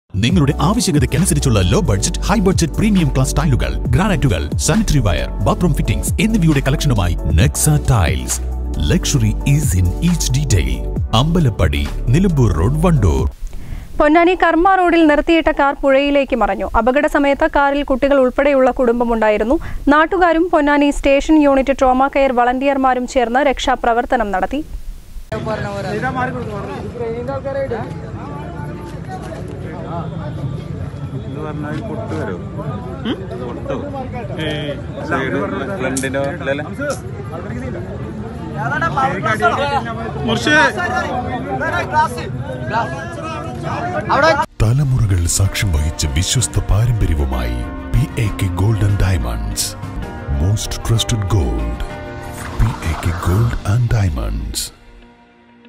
നിര്‍ത്തിയിട്ട കാര്‍ പുഴയിലേക്ക് മറിഞ്ഞു |SAHYA NOW| PONNANI
കാറില്‍ കുട്ടികള്‍ ഉള്‍പ്പെടെയുള്ള കുടുംബം ഉണ്ടായിരിന്നു.